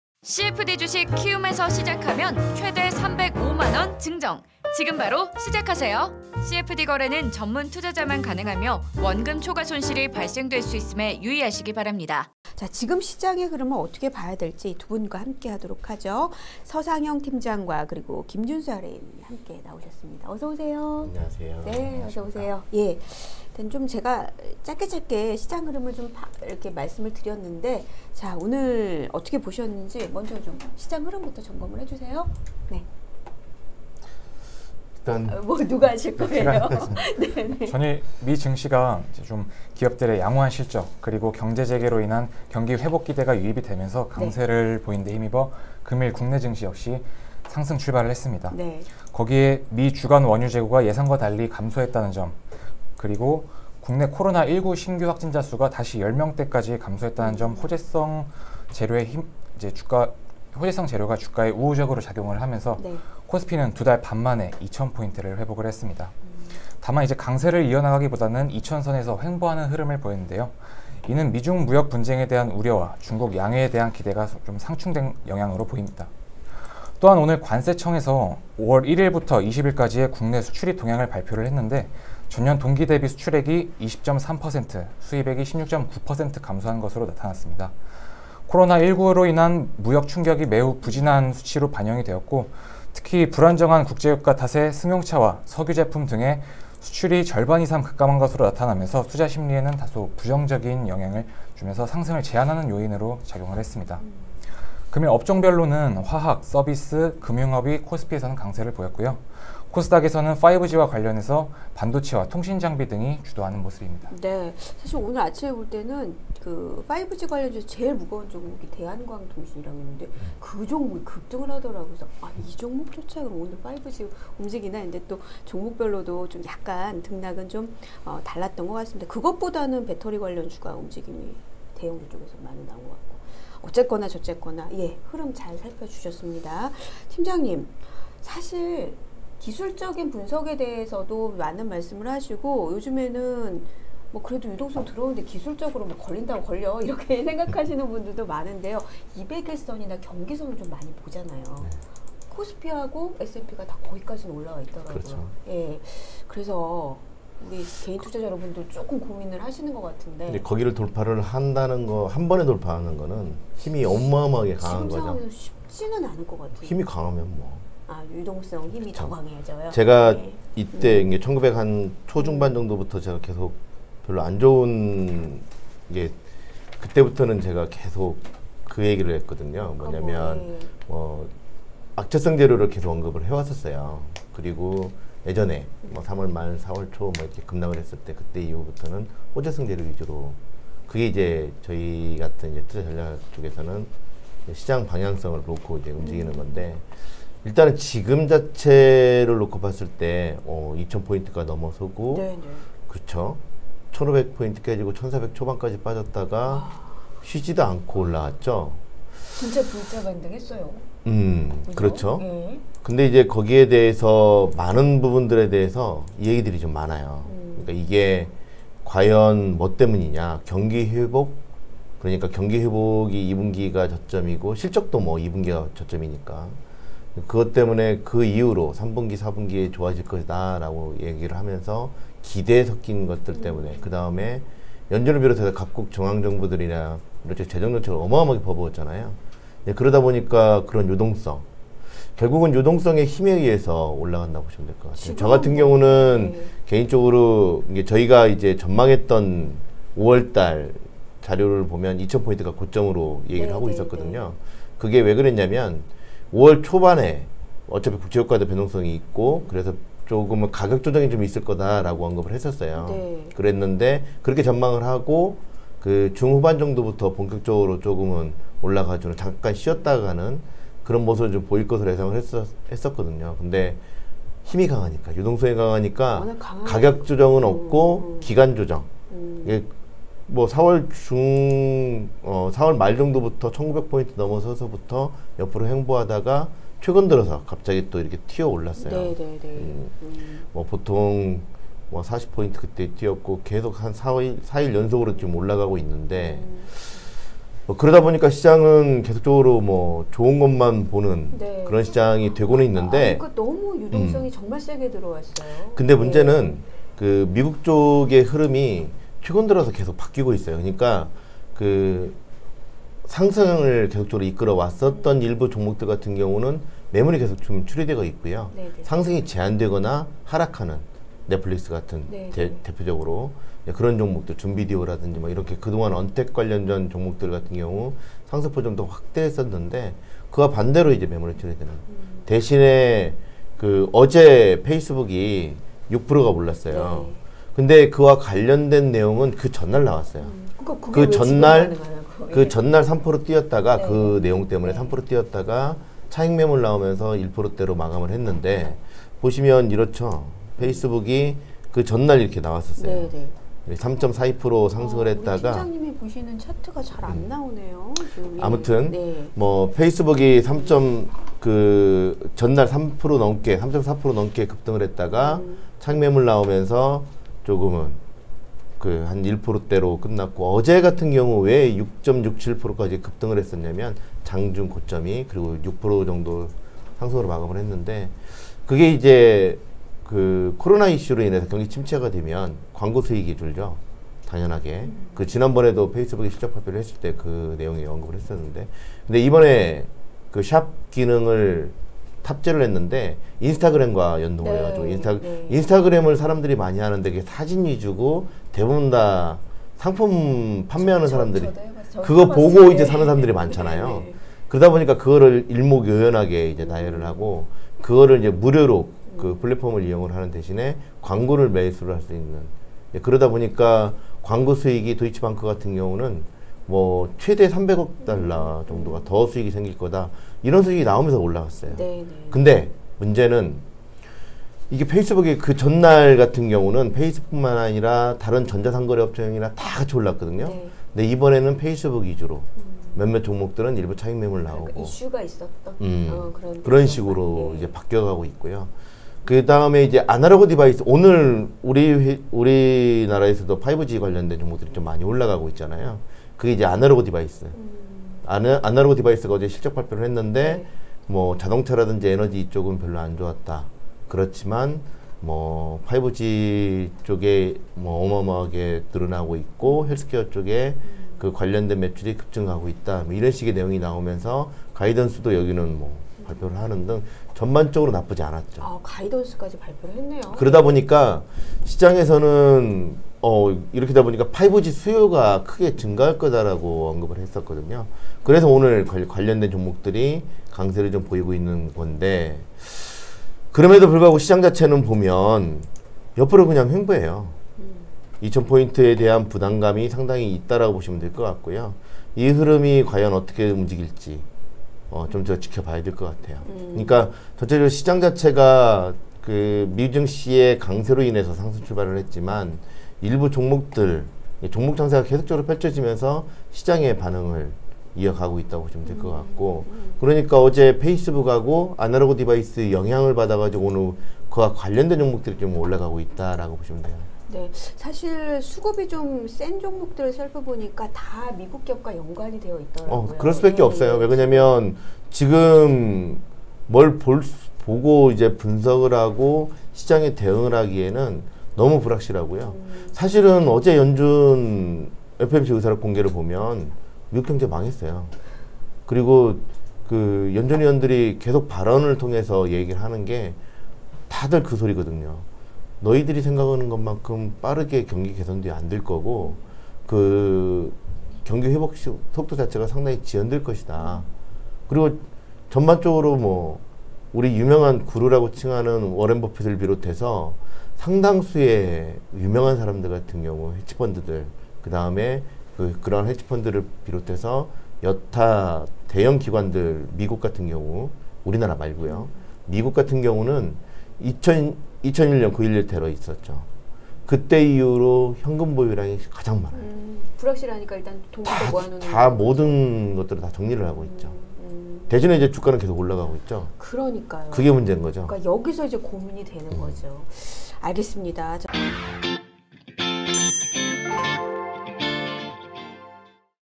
[주식투자][시장분석] 마감시황 /코스피 장중 2000선 터치, 코스닥 11개월만에 710선 탈환! (20.05.21)​
과연 경기회복 기대감에 의한 회복인가?
유동성의 힘에 의해서 상승하는 모습
​가격조정은 없고 기간 조정이 있는 모습
최근 미국 증시의 흐름은 상승했던 종목들은 매물이 출회되고 있는 모습
​페이스북 전일 6% 상승한 이유는 샵기능을 탑재하고 인스타그램과 연동이 가능한 기능을 선보였기 때문
전일 실적 발표한 아날로그 디바이스는 5G관련 매출이 급등하며 가이던스까지 양호.
​이러한 영향으로 국내증시도 5G관련주도 상승하는 모습
​시장은 미국 증시 강세로 동반 상승을 보였음​
김순영 채널K 앵커, 서상영 키움증권 투자전략팀장, 김준수 사원 키움증권 투자전략팀
「구독」과 「좋아요」 눌러주세요~